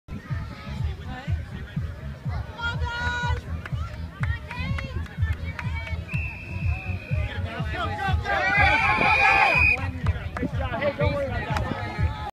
Irish @ Canes - Miles' great defense
Miles prevents 2pt pass by stepping in front of the receiver to intercept the ball.